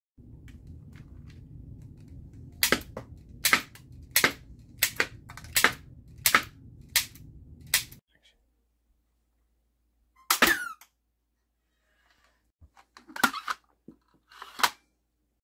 Topper Toys Johnny Eagle Red River Gun Set in Action!
This Toy set is mostly plastic and intended strictly as a toy with plastic non-firing ammunition; it can not be made to fire real ammo.